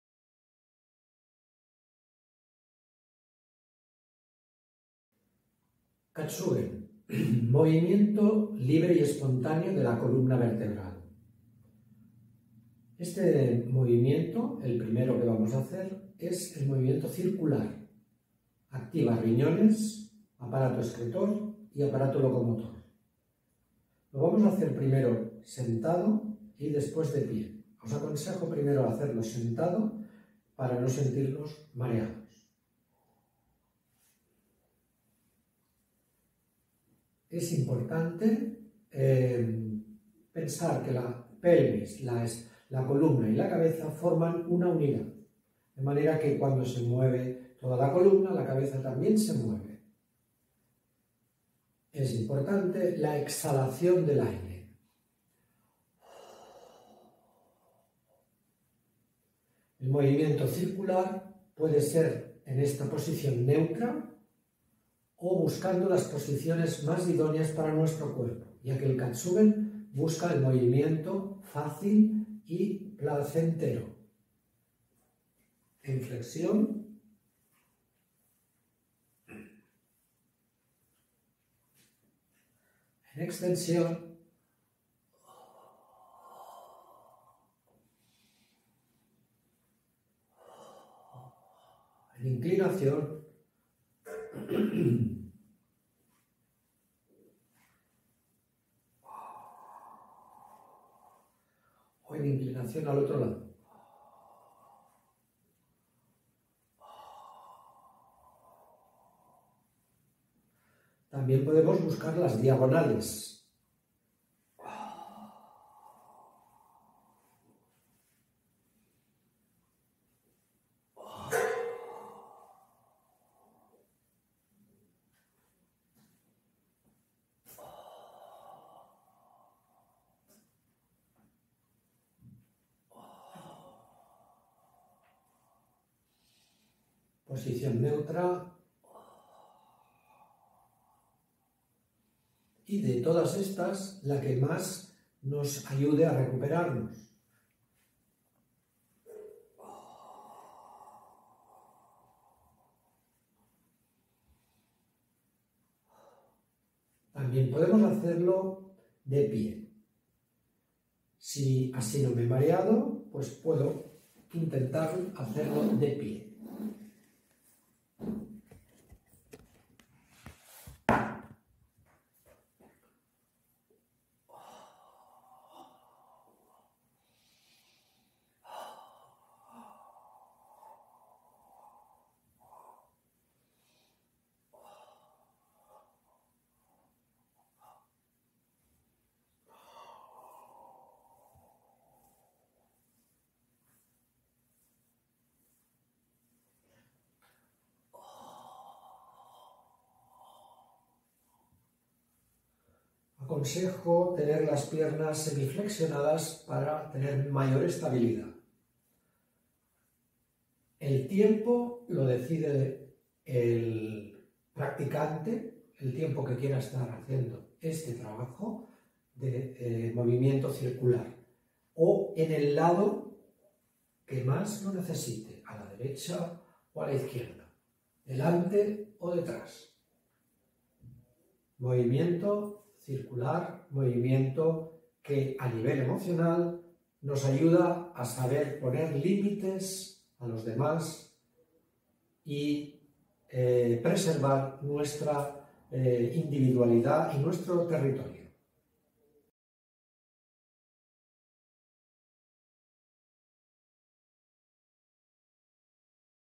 ¿Quieres iniciarte en el Katsugen? Aquí tienes el primer movimiento...
“El hombre está equilibrado mientras mantiene una buena relación con su propio cuerpo, con los otros y con su entorno, la naturaleza. Cuando el hombre pierde cualquiera de esas relaciones pierde su equilibrio.”

El Seitai fue fundado por el Sensei Haruchika Noguchi. La Fundación SEITAI promueve la regulación del cuerpo a través de tres técnicas muy sencillas, entre las que está el Katsugen que es la gimnasia corporal instintiva.

Aquí tienes el primer movimiento que es el giro o la torsión.

🧐 Puedes leer más sobre el Seitai en la siguiente entrada del blog